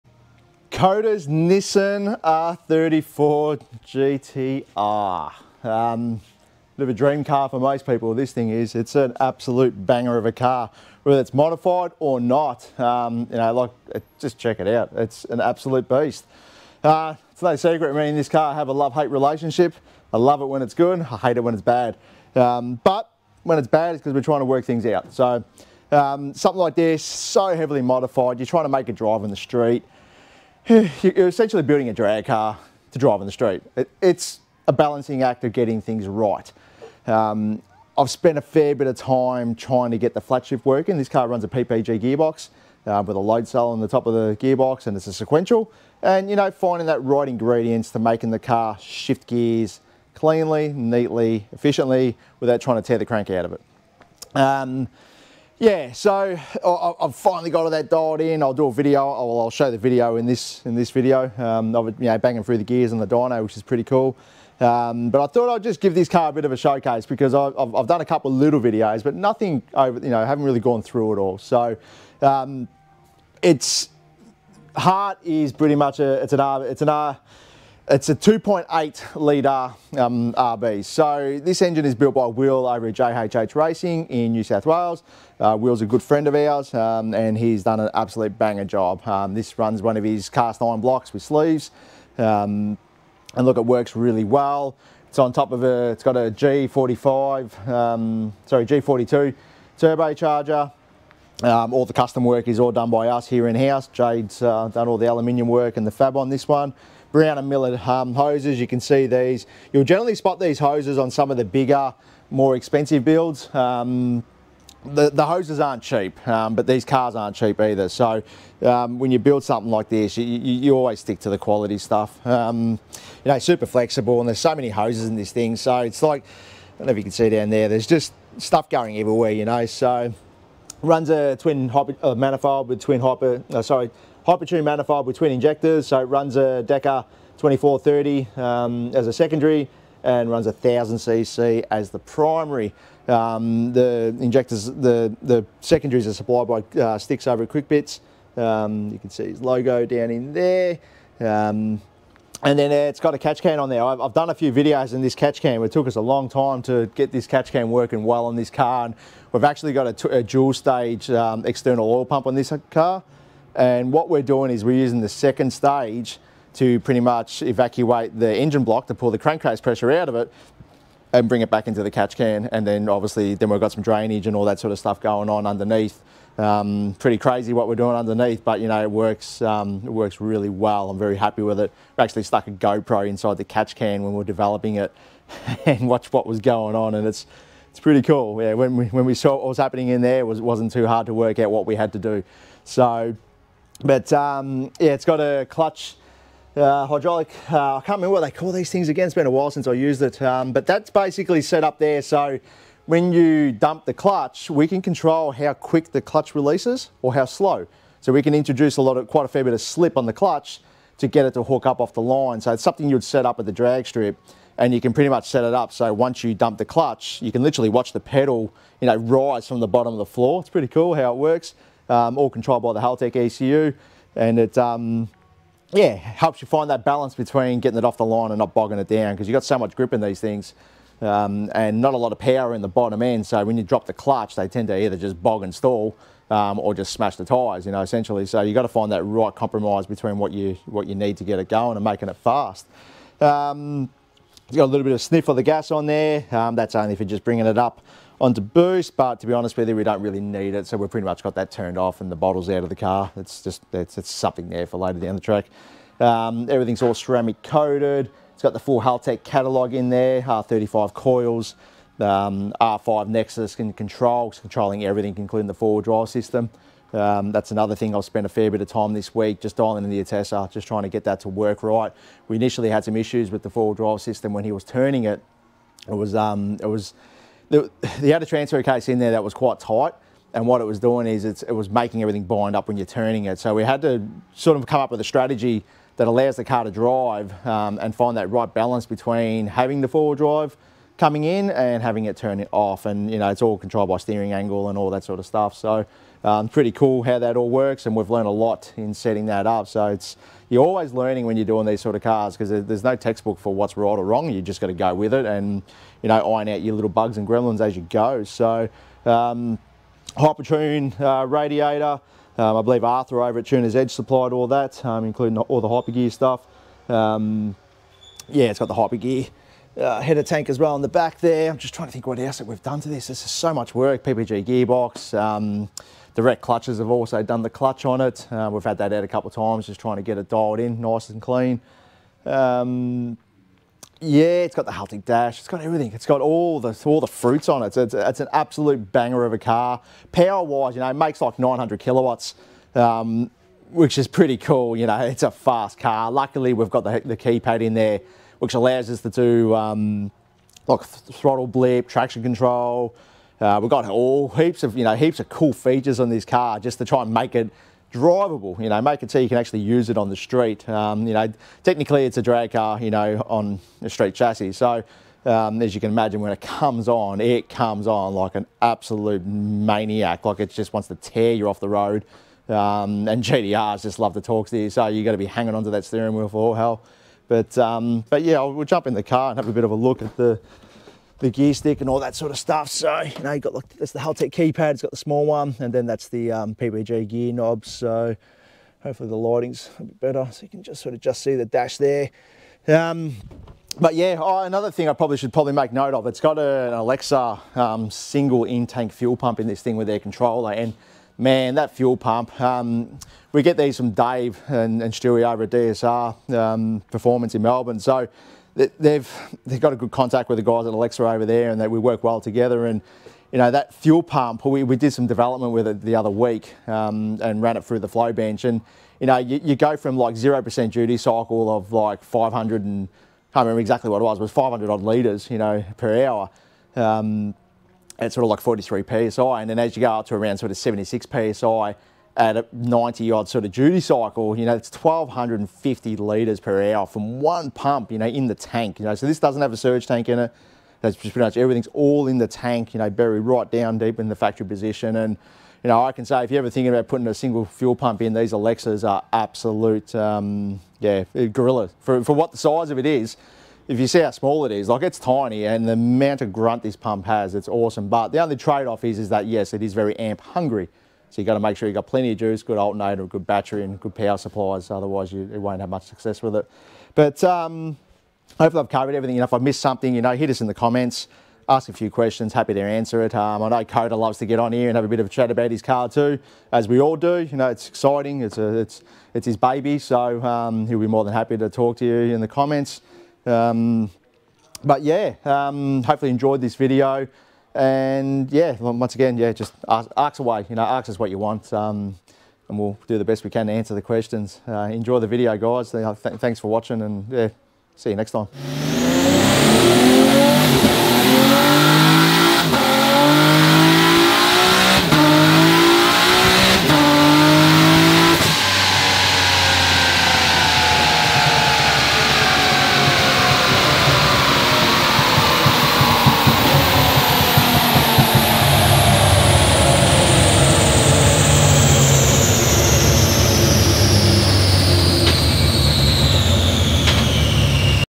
Skyline GTR Build: Transmission Upgrades for Ultimate Performance
🏎️ Skyline GTR Build: Transmission Upgrades for Peak Performance 🏎️ This build isn’t just about power – it's about making sure your Skyline GTR can handle it all! Watch as we dive into the crucial transmission upgrades that take this iconic machine to the next level. From precision shifts to improved durability under extreme conditions, we’ve got it all covered. Ready to transform your ride? 🚗💨

🔗 Follow Us & Shop with Us! 🔗